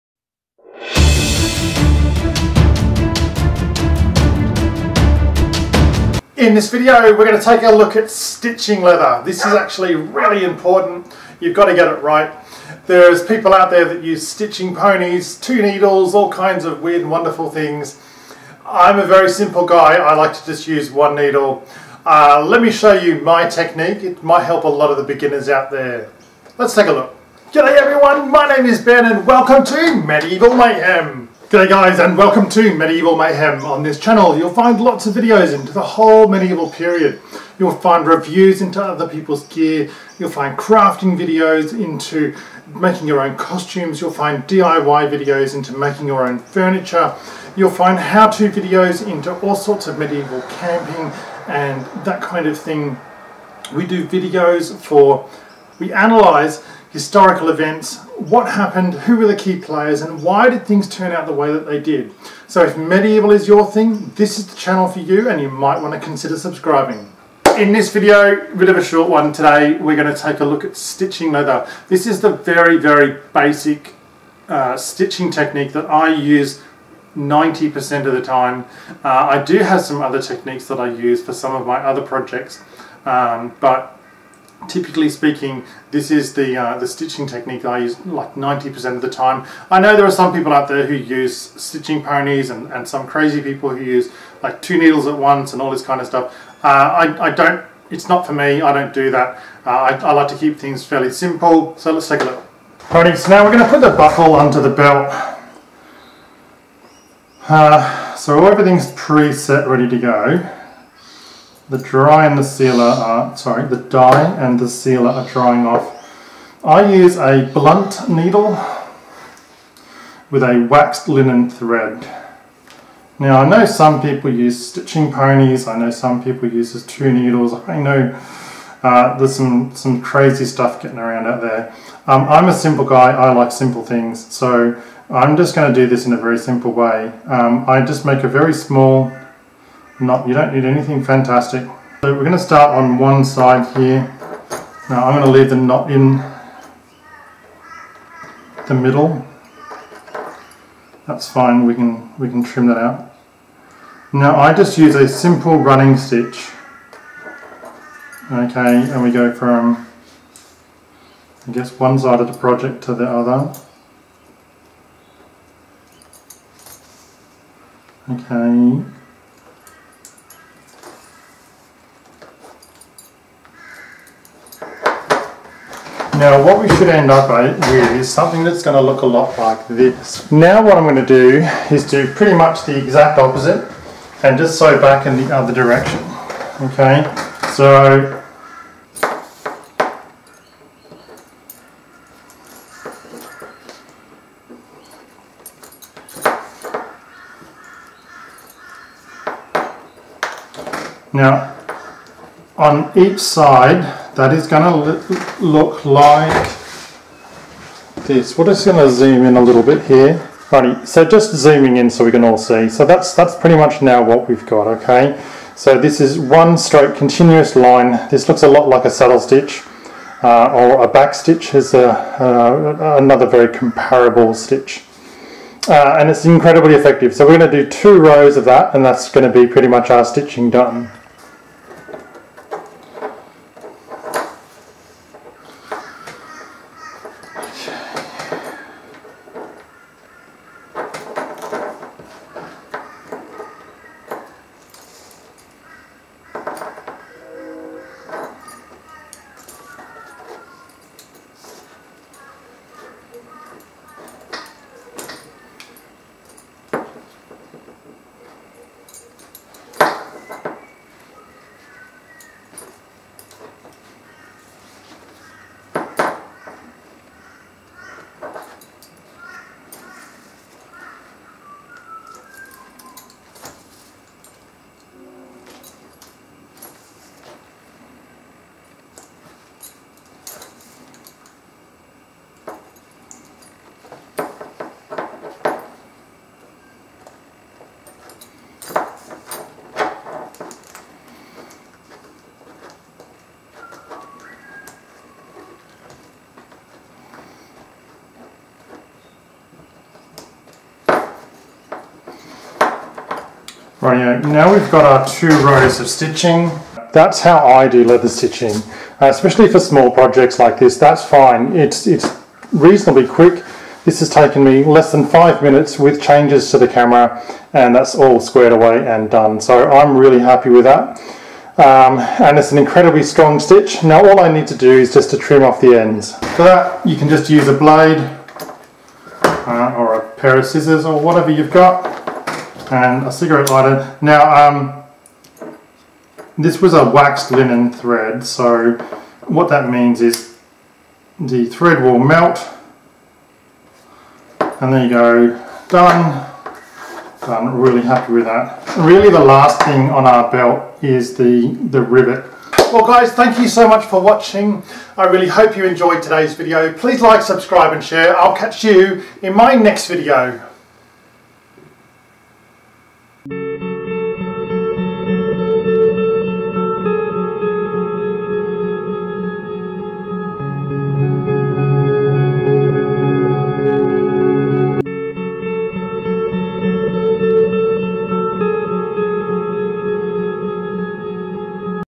How to Stitch Leather (the easy way)
In this video we look at how to stitch leather projects.